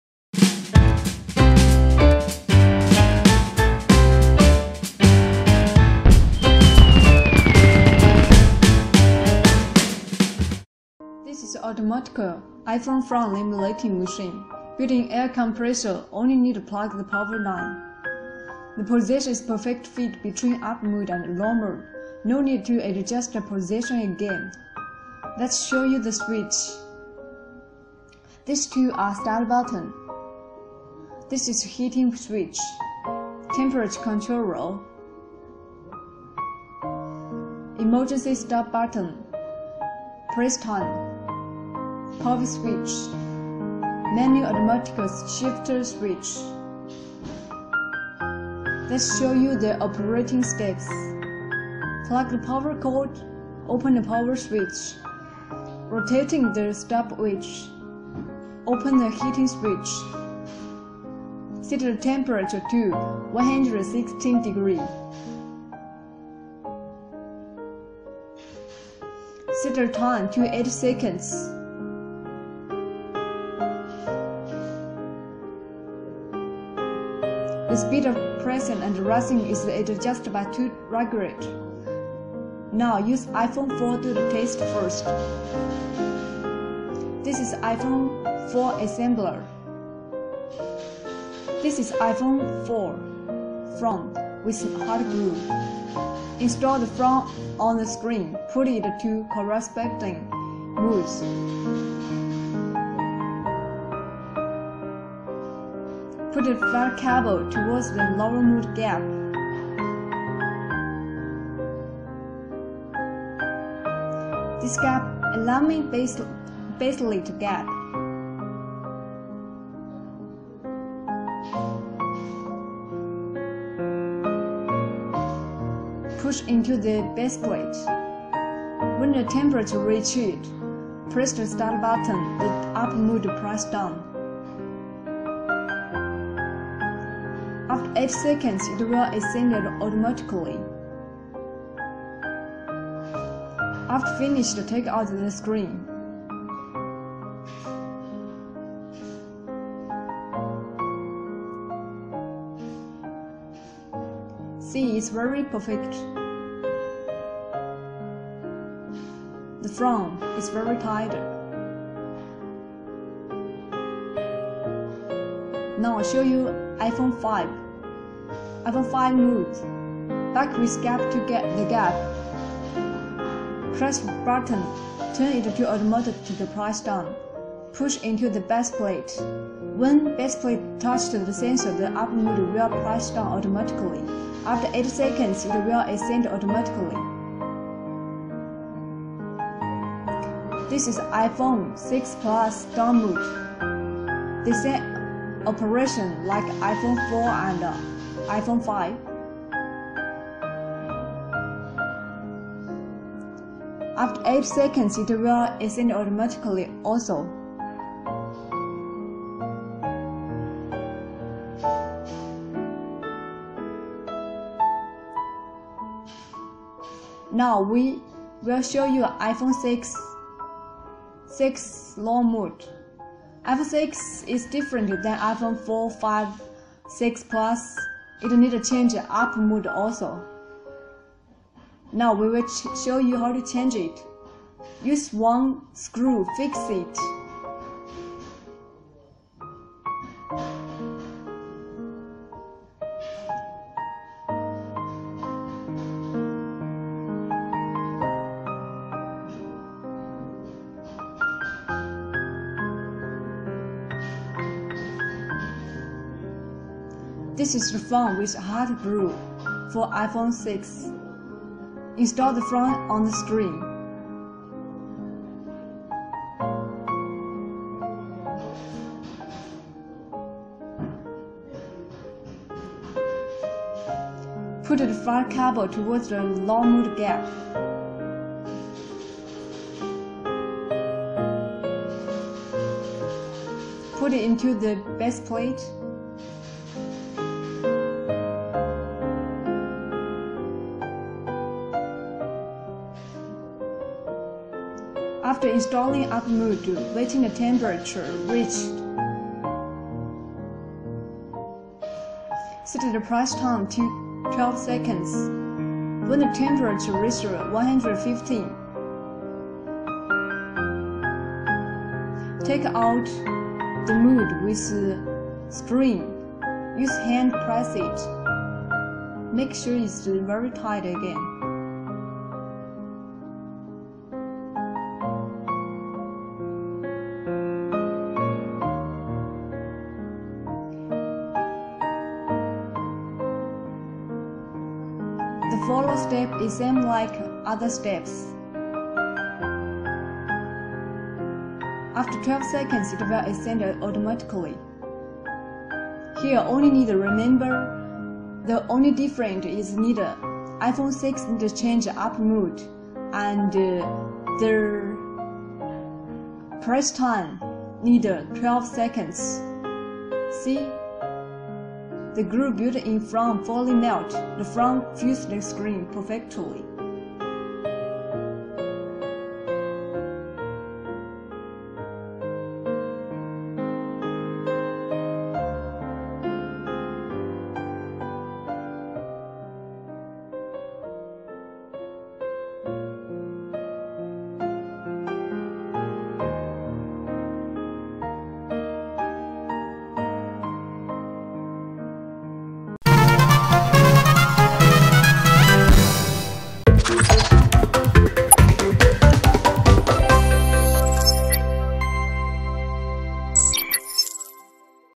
Professional Automatic Frame Laminator Bracket Pressure Laminating Machine for iPhone with 5 Moulds
Features:
This is an automatic frame laminating machine with built-in air compressor.
Come with 5 phone moulds for iPhone, including 4 lower moulds and 1 upper mould.
4 lower moulds: 4/4s, 5/5s/5c, 6(4.7), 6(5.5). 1 upper mould: 6(4.7).
The upper mould is only needed for iPhone 6 frame pressing and it is easy and convenient to install.
2 ways to press: manual press and auto press. Press the left and right start button together for manual press mode, open the automatic switch on the side for automatic press mode.
With this machine, you can install the frame of phones easily and quickly.